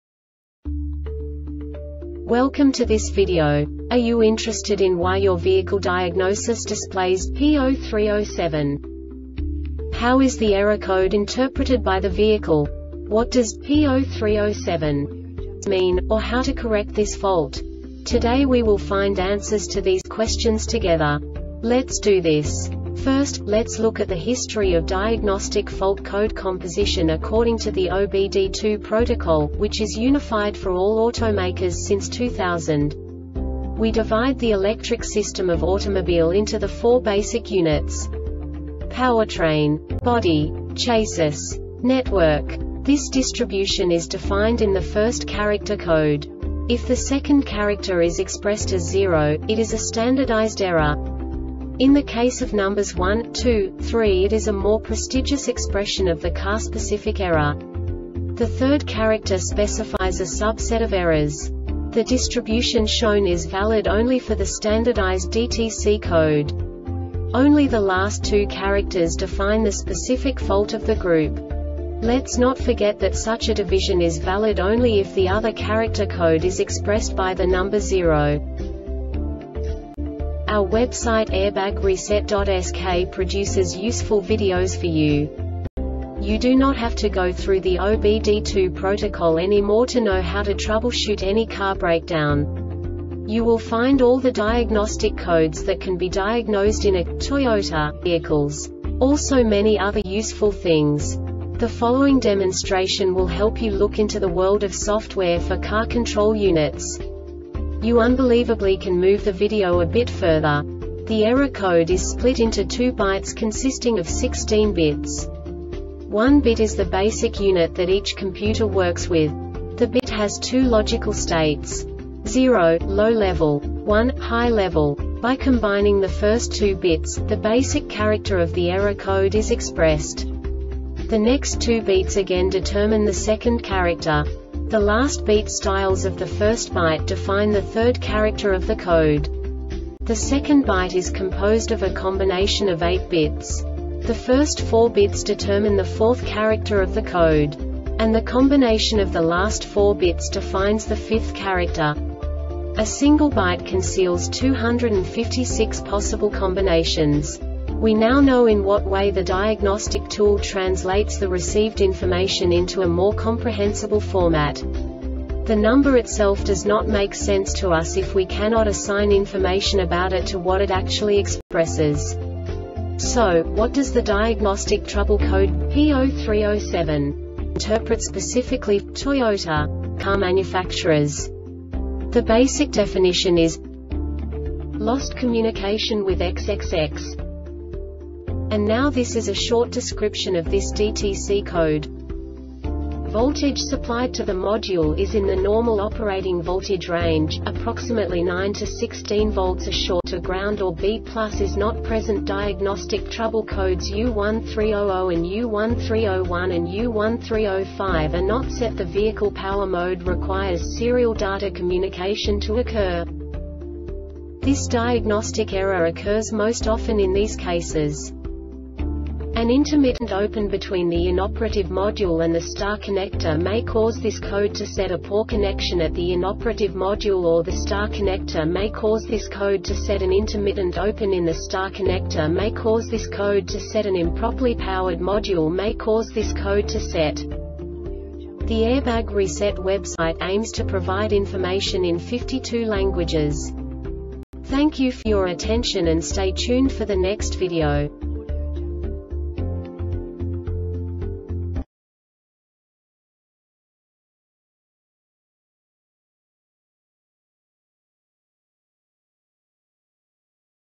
DTC Toyota P0307 Short Explanation
Contents:
0:21 Basic DTC analysis according to OBD2 protocol standard.
1:48 Insight into programming
2:58 A diagnostically understandable description of the Diagnostic Trouble Code
3:05 Basic error definition

Make:
Toyota
Code:
P0307
Definition:
Lost Communication with XXX
Description:
Voltage supplied to the module is in the normal operating voltage range,approximately 9-16 volts. A short to ground or B+ is not present.Diagnostic trouble codes U1300 and U1301 and U1305 are not set. The vehicle power mode requires serial data communication to occur.
Cause:
 An intermittent open between the inoperative module and the star connector may cause this code to set. A poor connection at the inoperative module or the star connector may cause this code to set. An intermittent open in the star connector may cause this code to set. An improperly powered module may cause this code to set.